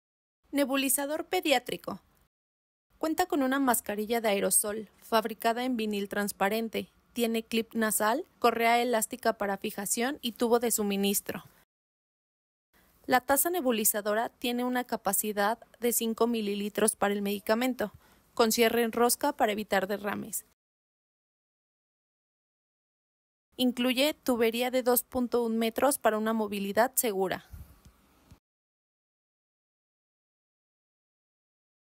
Kit Nebulizador Pediátrico con Mascarilla y Tubería de 2.1 Metros.
El kit nebulizador pediátrico incluye: una mascarilla pediátrica, taza nebulizadora y tubo de aire de 2.1 metros. Es de uso desechable.